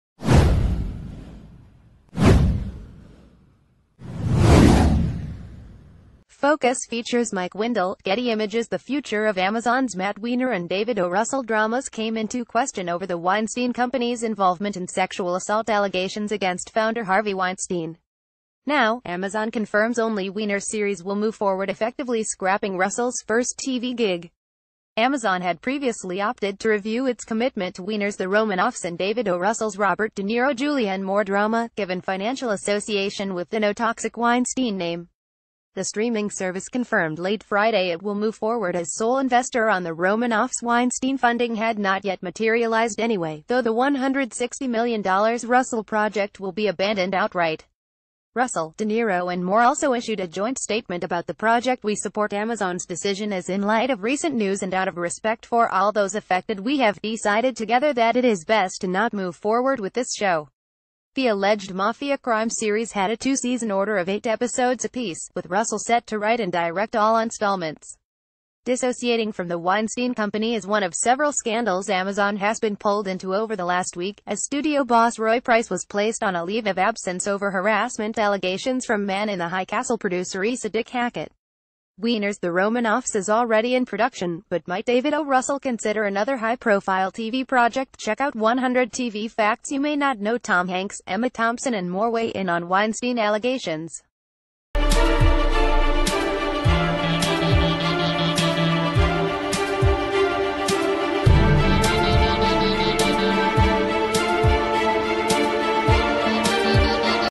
Breaking News | Amazon scraps david o. russell tv series over weinstein ties
Amazon scraps david o. russell tv series over weinstein ties

Focus Features Mike Windle, Getty Images The future of Amazon’s Matt Weiner and David O. Russell dra...